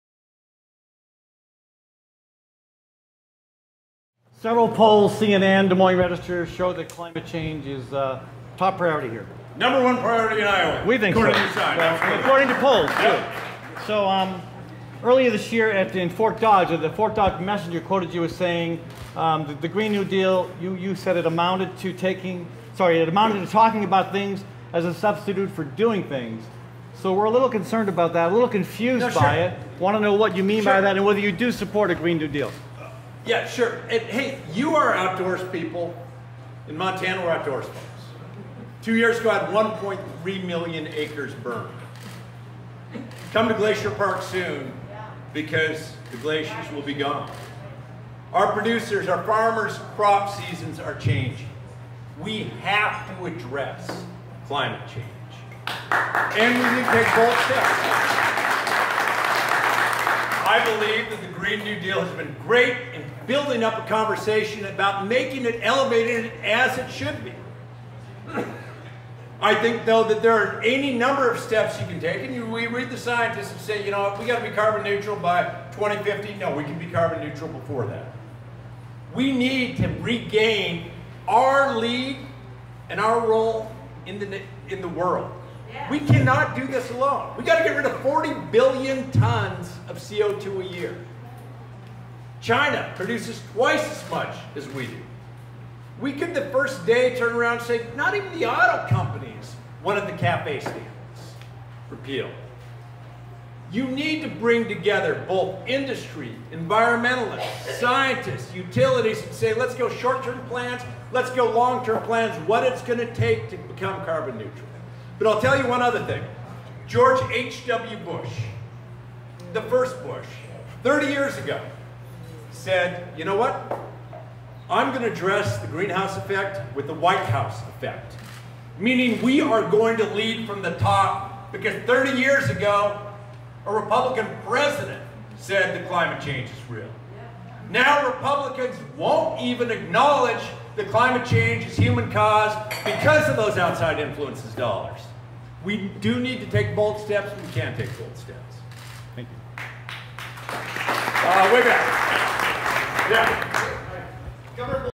Bold Iowa Climate Bird Dogs and Steve Bullock, 5-16-19 in Des Moines, Iowa
To join us in bird-dogging or see other candidates' responses, go